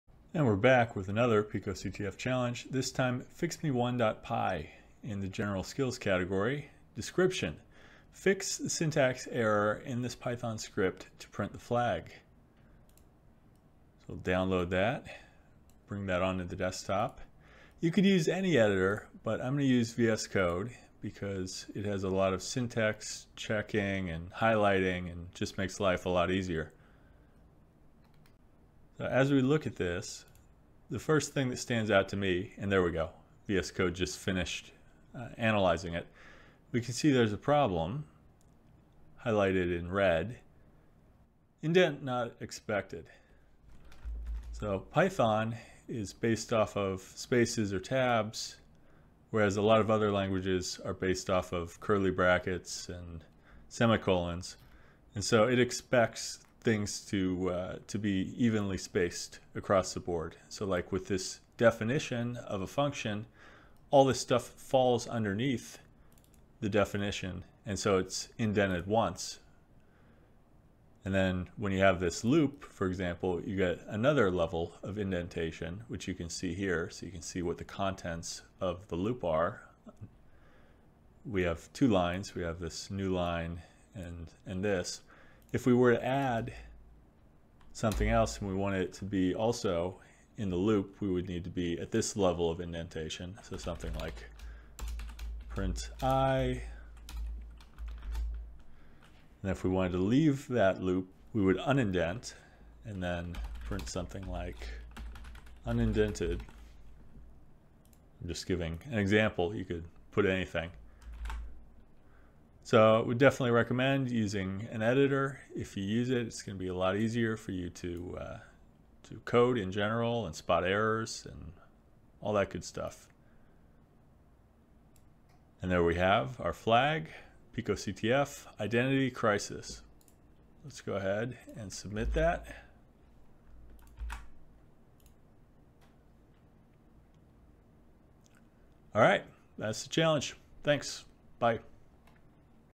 PicoCTF Walkthru [72] - fixme1.py
Running through the CMU PicoCTF. Analysis and walkthrough of the challenge "fixme1.py"